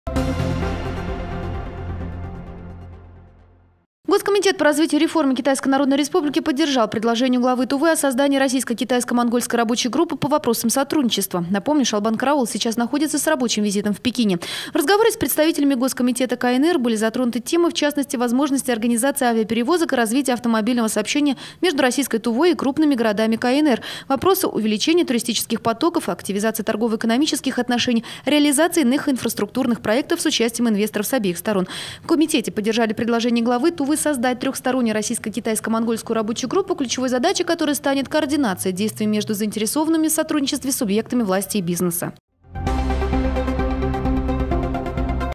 ПОЛИТИКА - Авиаперевозоки развития автомобильного сообщения между Тувой и КНР
Напомню, Шолбан Кара-оол сейчас находится с рабочим визитом в Пекине.